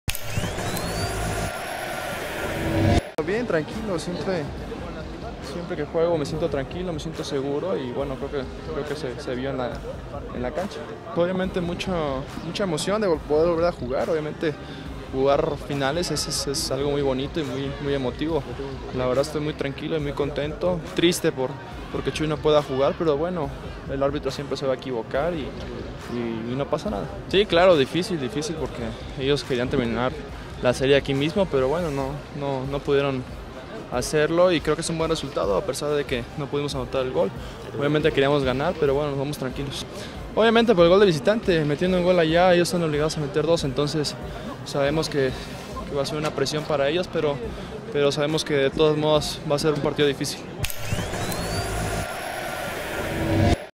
Estoy listo para lo que viene: Guillermo Allison
Guillermo Allison se dijo listo para afrontar lo que viene y descartó haberse sentido nervioso o presionado ante la responsabilidad de resguardar el marco celeste